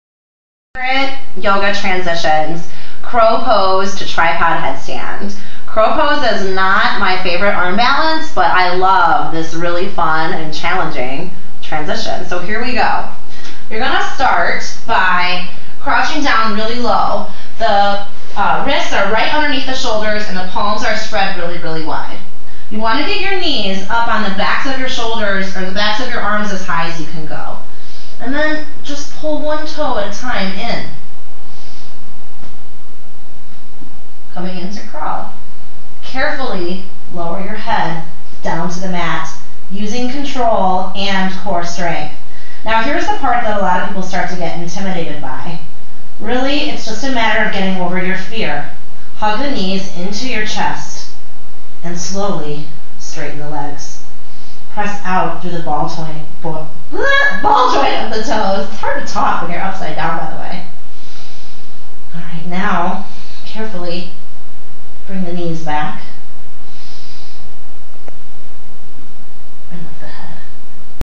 Yoga Moves: Crow pose to tripod headstand.mp4
How to go from the basic arm balance, crow pose, to tripod headstand.  This is a fun and challenging series that works your core and invigorates from within.